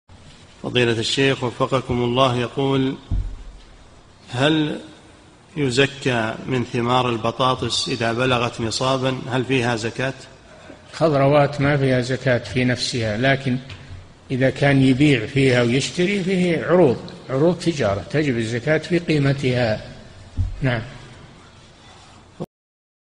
[1083 -3022] هل في ثمار البطاطس زكاة؟ - الشيخ صالح الفوزان
-"[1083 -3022] السؤال: يقول: هل يزكى من ثمار البطاطس إذا بلغت نصابا؟ هل فيها زكاة؟
الجواب: الجَوَابُ: الخضروات ما فيها زكاة في نفسها لكن إذا كان يبيع فيها ويشتري فيهي عروض، عروض تجارة تجب الزكاة في قيمتها، نعم.
للاشتراك في قناة الشيخ صالح الفوزان

للاستفسار والإقتراحات والدعم
رئيس الفريق
لمشروع كبار العلماء